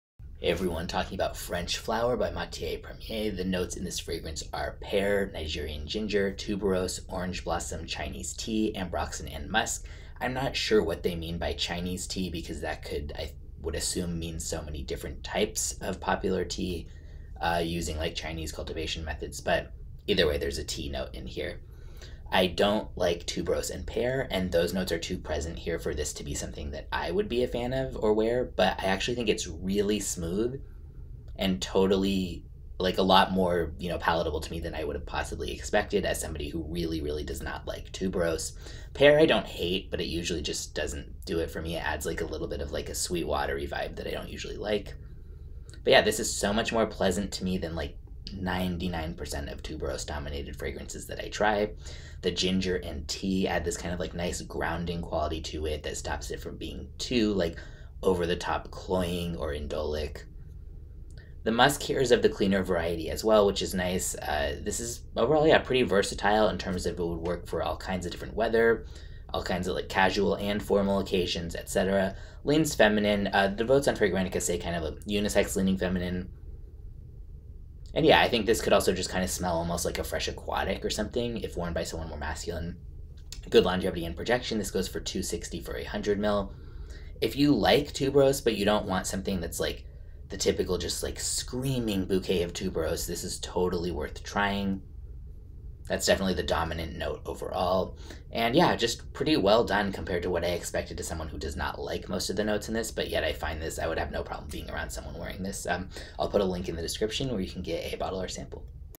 Matiere Premiere French Flower Review | Tuberose/Pear/Ginger/Orange Blossom/Tea | Unisex/Feminine
Review of French Flower by Matiere Premiere.

The notes in this fragrance are:

Pear, Nigerian ginger, tuberose, orange blossom, Chinese tea, Ambroxan, Musk